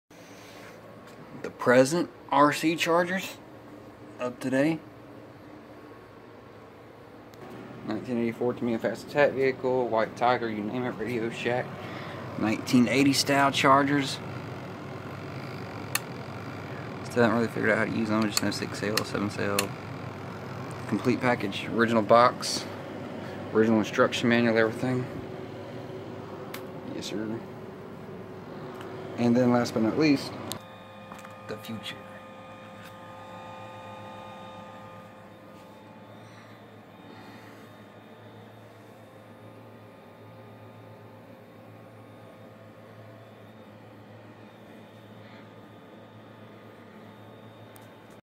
present past and future of RC car chargers
the 1980s one is pretty sick and cool but I'm going to tell you what those new Fast Chargers are really awesome can charge the battery and less than 20 minutes even a 25c 11.1v and 12.7v CRAZY